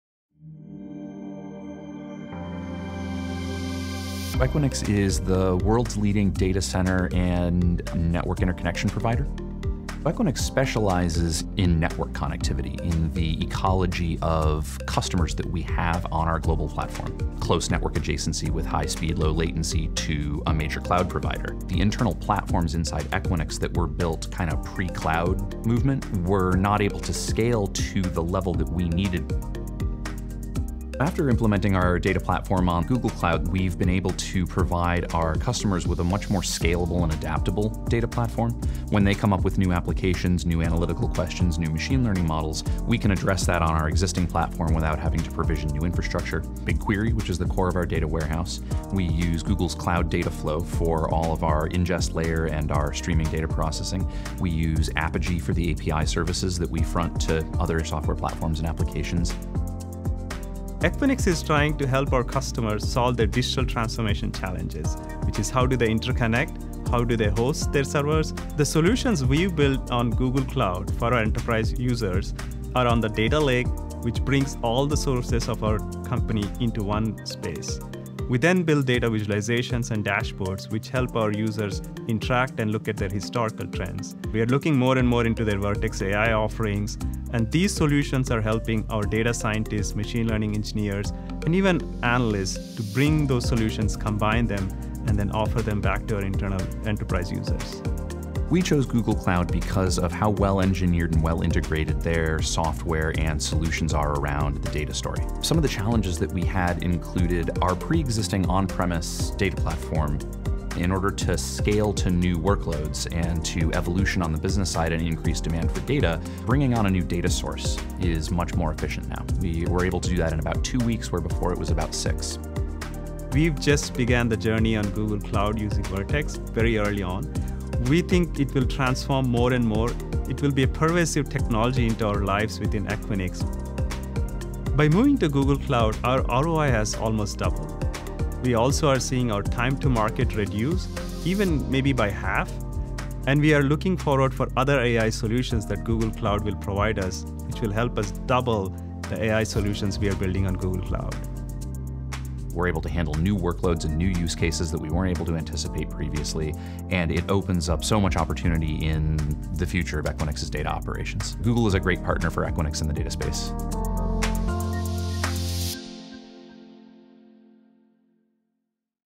Equinix Drives Strategic Value With AI & ML Solutions From Google Cloud
Learn how Equinix, a leading global data center and network interconnection provider, leveraged Google Cloud’s AI solutions to double ROI and cut time to market in half for its internal IT operations teams.